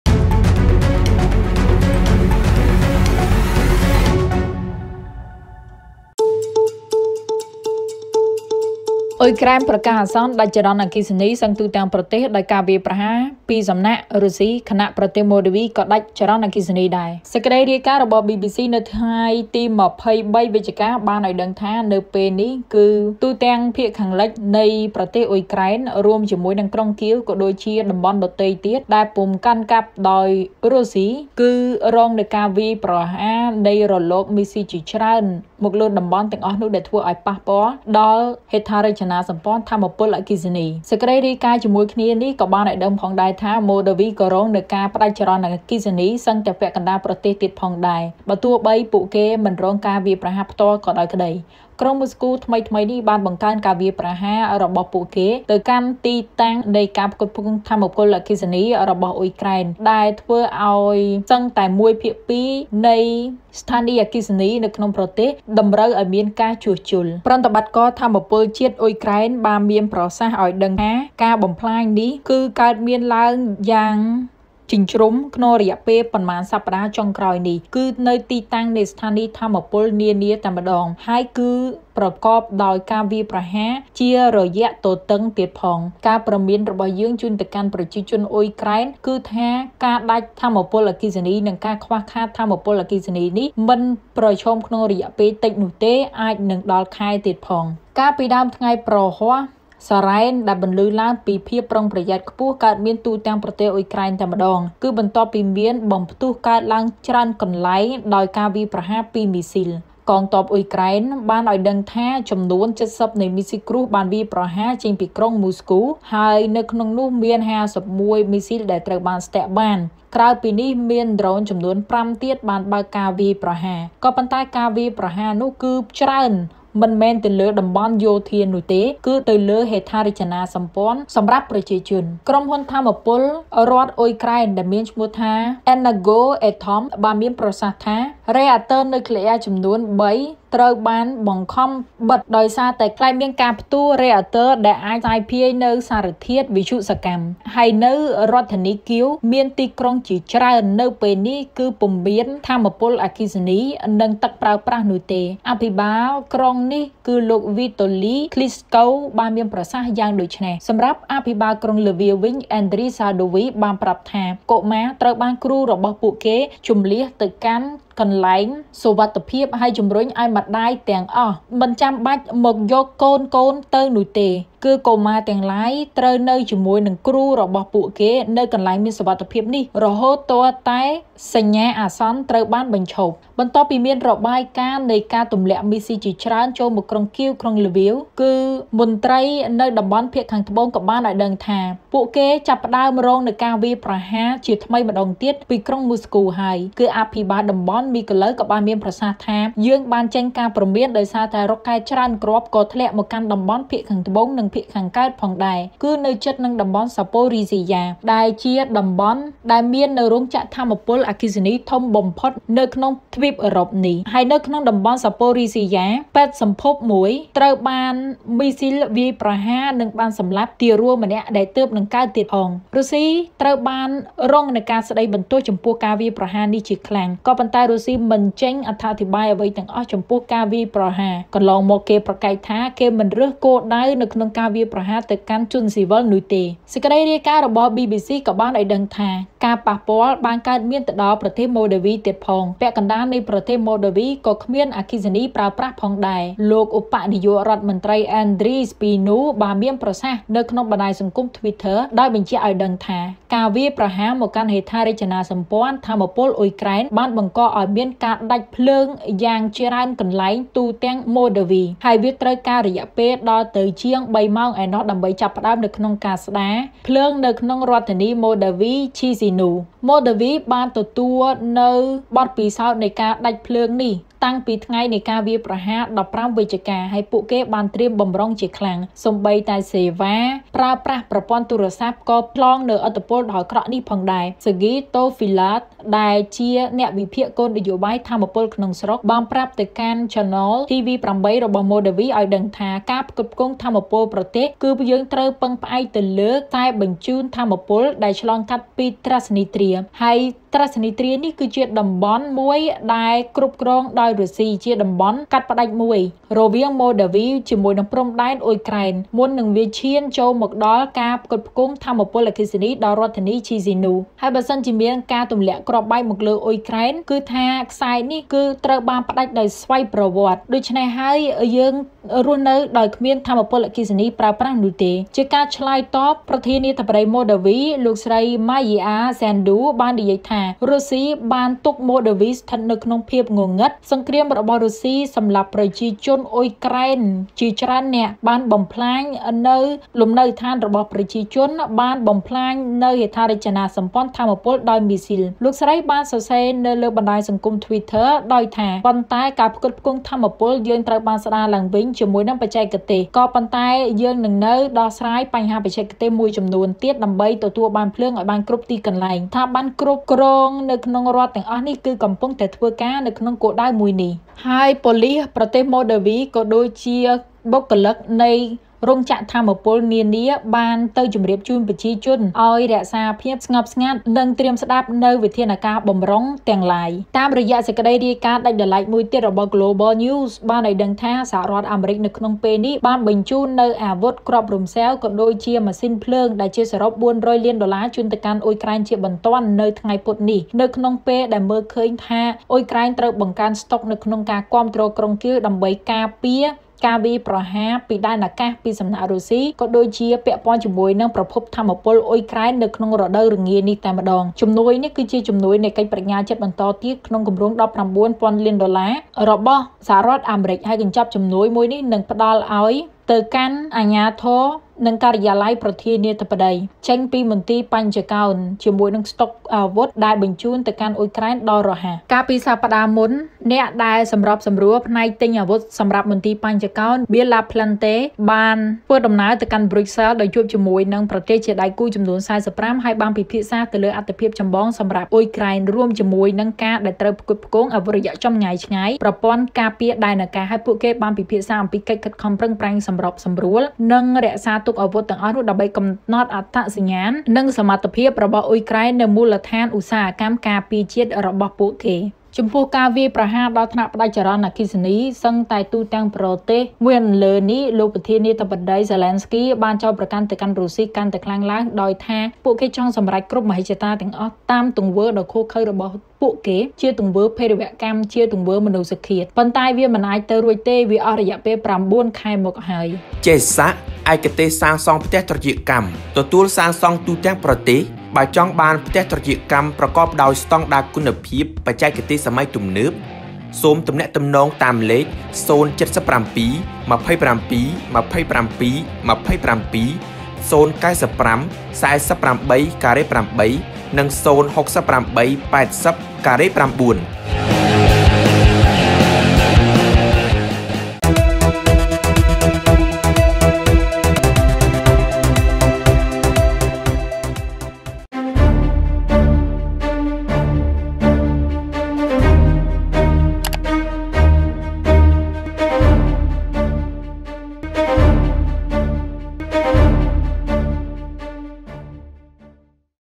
អ៊ុយក្រែនប្រកាសអាសន្នដាច់ចរន្ដអគ្គិសនីសឹងទូទាំងប្រទេសដោយមីស៊ីលរុស្ស៊ីវាយប្រហារ
A barrage of Russian missile strikes has killed 10 people in Kyiv and left much of the capital without power; Zelenskyy calls for "very firm action" from the rest of the world in response; listen to the latest episode of Ukraine War Diaries as you scroll.